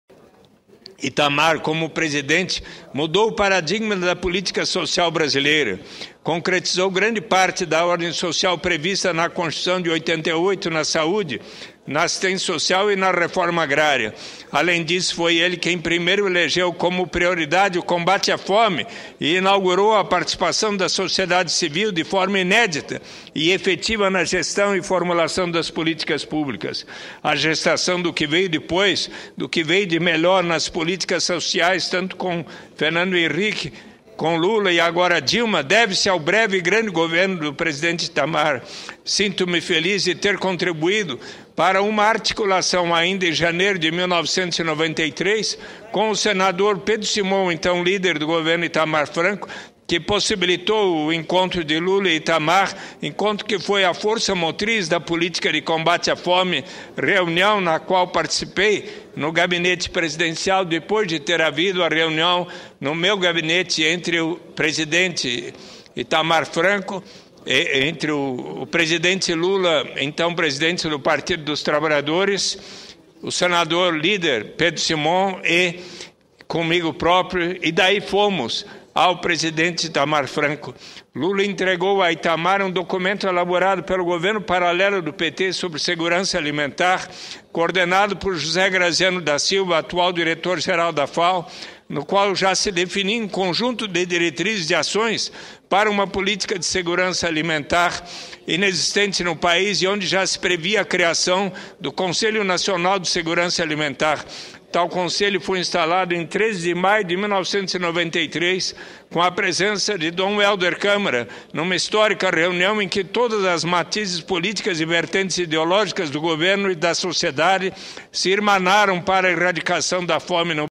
"Foi ele quem primeiro elegeu como prioridade o combate à fome", diz sen. Suplicy sobre Itamar
05/07/2011 - Plenário Discursos - "Foi ele quem primeiro elegeu como prioridade o combate à fome", diz sen. Suplicy sobre Itamar